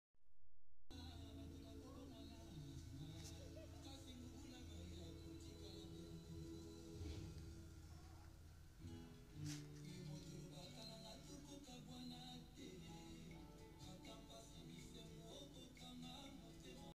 print on demand Kenneth Kaunda design print - Clothing Art
The print on demand hand painted T-shirt design was inspired by the late first Republican President of Zambia Dr Kenneth David Kaunda. It features his head and wildlife motifs and the famous land mark skycraper that dominates the Lusaka city's sky line.